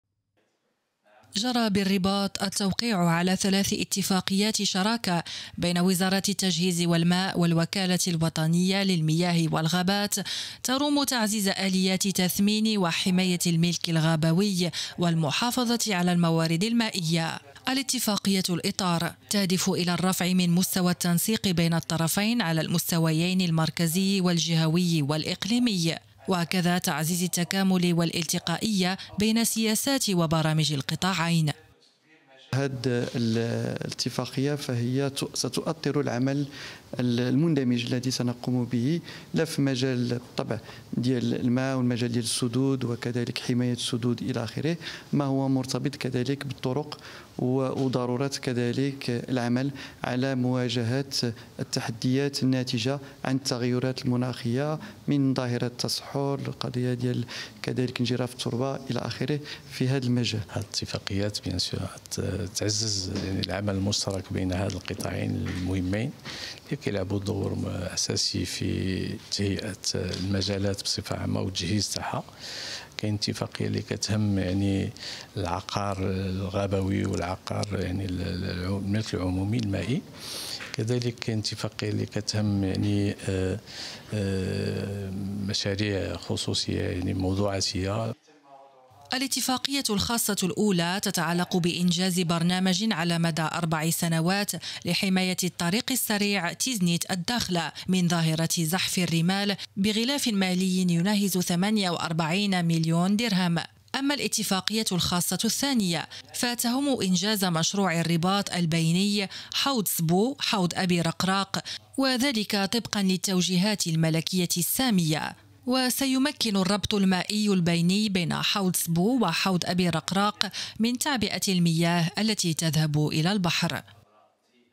الرباط.. التوقيع على اتفاقيات شراكة لحماية الملك الغابوي والمحافظة على الموارد المائية
جرى  بالرباط، التوقيع على ثلاث اتفاقيات شراكة، بين وزارة التجهيز والماء، والوكالة الوطنية للمياه والغابات،تروم تعزيز آليات تثمين وحماية الملك الغابوي والمحافظة على الموارد المائية.

لمتابعتنا

لتحميل التطبيق